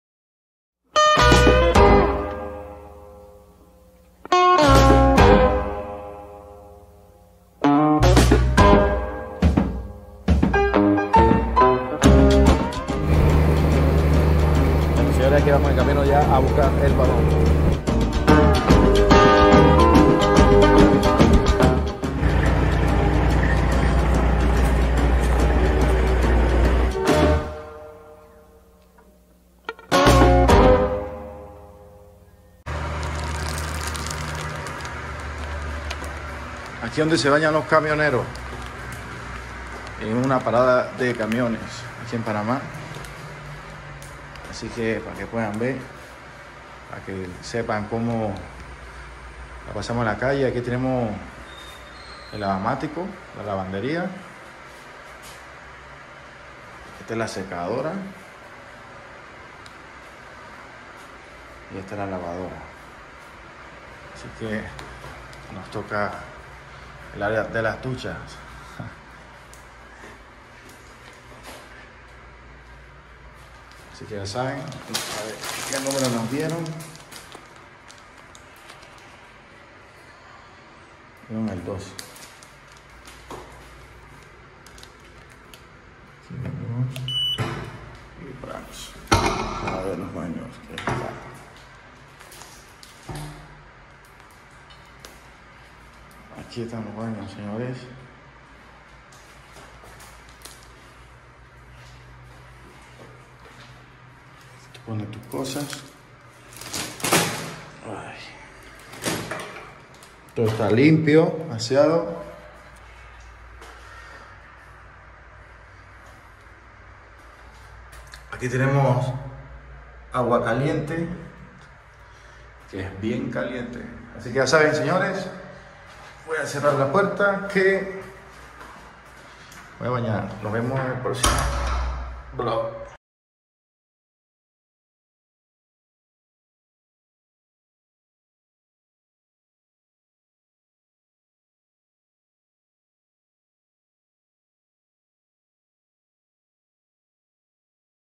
Baños para camioneros /Aquí es donde se bañan los camioneros(DUCHAS)/ como es las vida de camioneros
Video de aquí es donde se bañan los camioneros en una parada de camiones en panama para enseñarles las paradas donde lavamos ropas comemos llenamos diésel etz saludos espero les guste subscríbete  mundo de camiones rutas aventuras cargas precios.